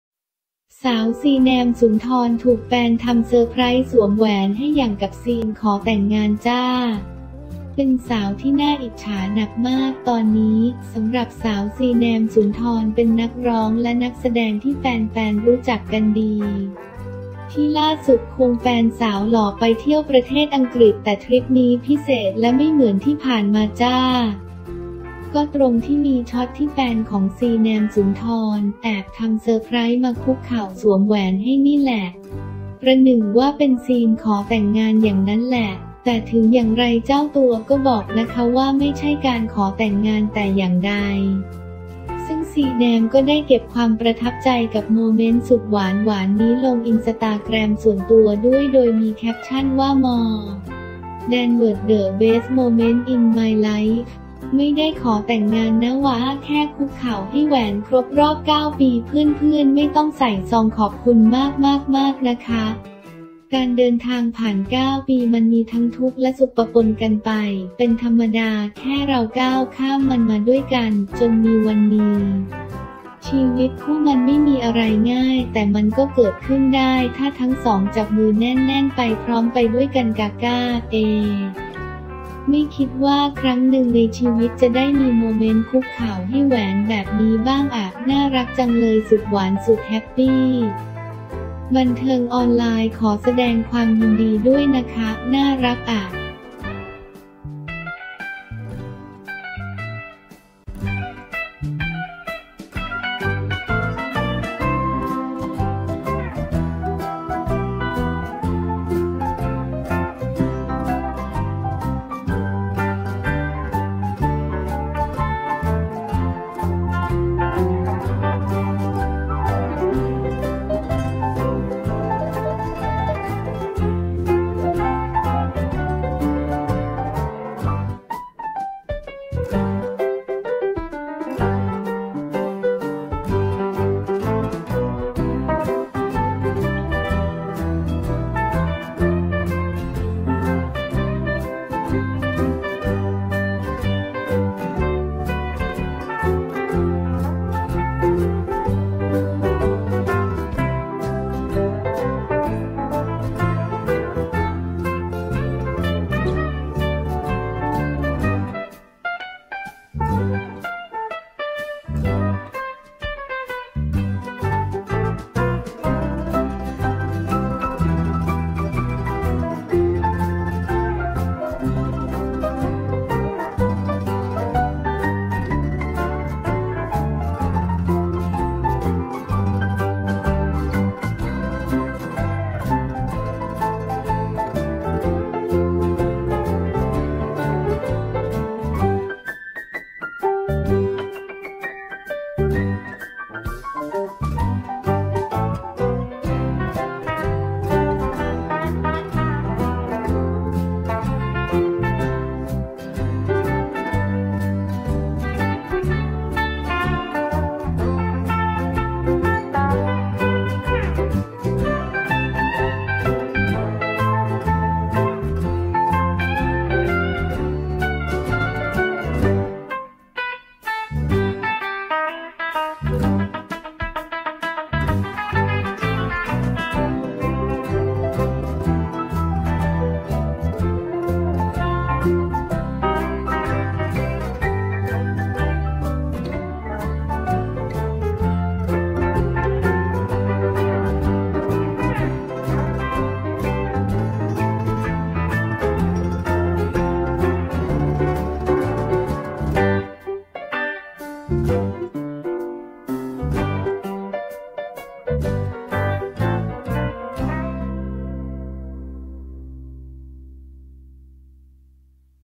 ซีแนม สุนทร ถูกแฟนสาวหล่อคุกเข่าให้แหวน เป็นสาวที่น่าอิจฉาหนักมากตอนนี้ ชีวิตคู่ถ้าเราจับมือกันแน่นๆ
ซีแนม สุนทร ถูกแฟนสาวหล่อคุกเข่าให้แหวน เป็นสาวที่น่าอิจฉาหนักมากตอนนี้
 ชีวิตคู่มันไม่มีอะไรง่าย แต่มันก็เกิดขึ้นได้ถ้าทั้งสองจับมือแน่นๆไปพร้อมไปด้วยกัน...